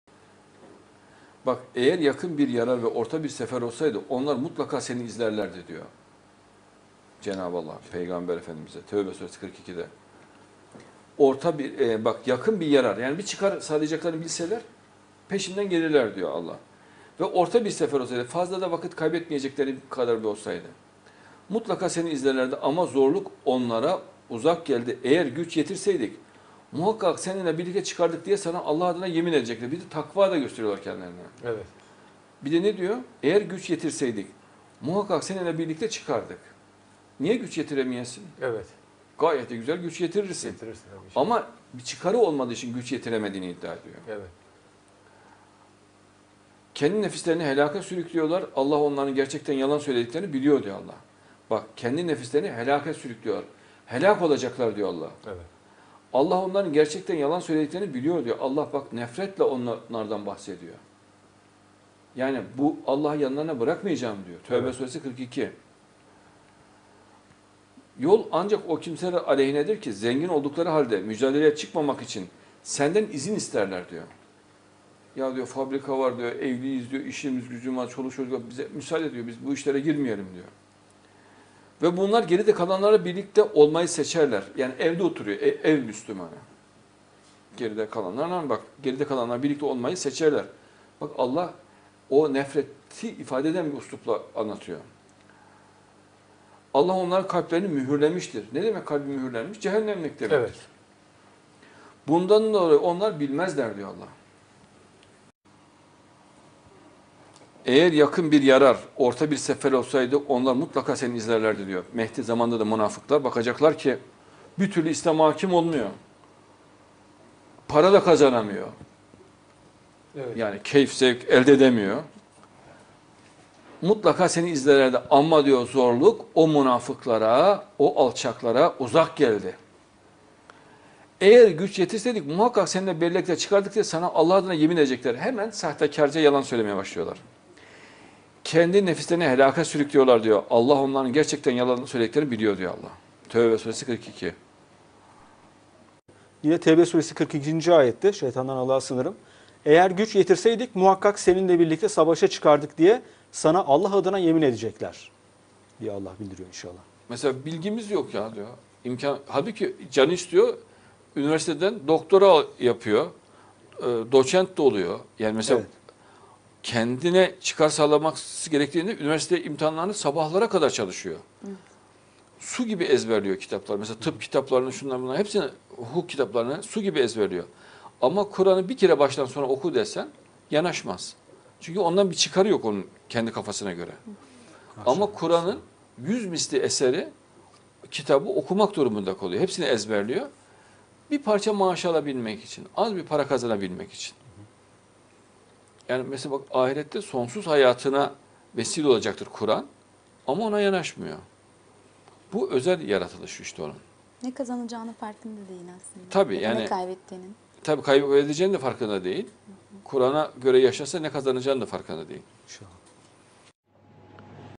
Ruhsuz Münafıklar » Hayvanın baktığı gibi bakıyor, duyuyor, öttüğü gibi konuşuyor..
Şeytanın çirkin fısıltısını, münafıkları dinlemek Kuran’a göre haramdır. Hiçbir münafığın, hiçbir şeytanın, İslam aleyhine söylenen hiçbir sözün dinlenmemesi gerekir. Allah, İslam aleyhine konuşma olduğunda oradan uzaklaşmak gerektiğini bildirmiştir. Müslüman, münafığın pisliğiyle, lağım gibi dünyasıyla muhatap olmaz. (Adnan Oktar, 3 Temmuz 2018)